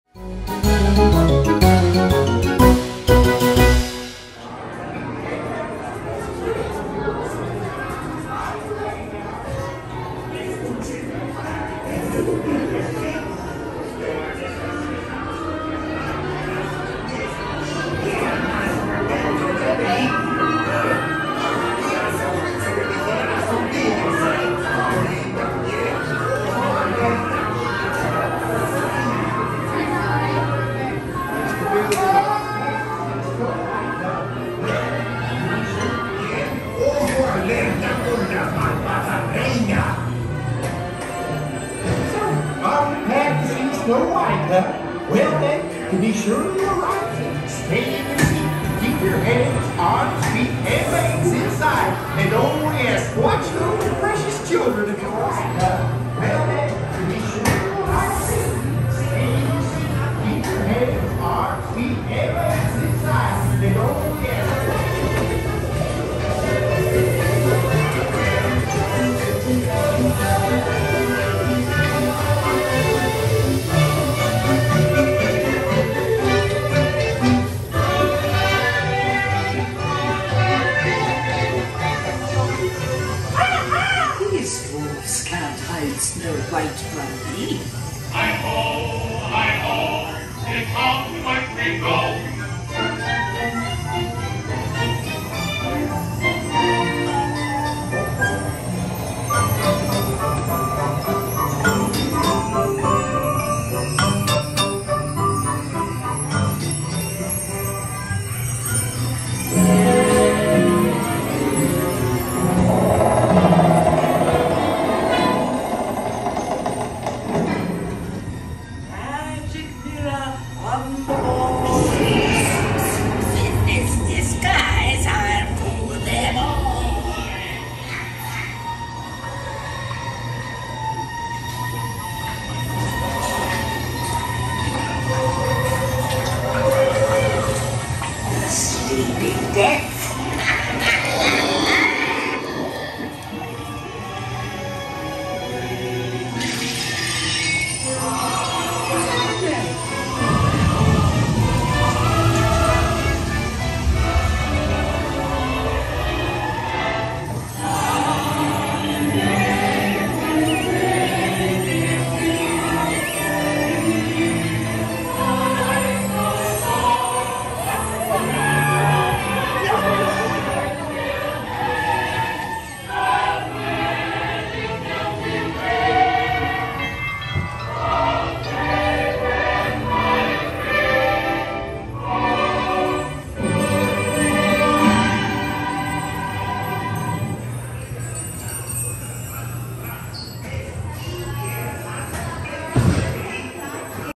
Snow White's Enchanted Wish Full Ride
Full ride of Snow White's Enchanted Wish at Disneyland in Anaheim, CA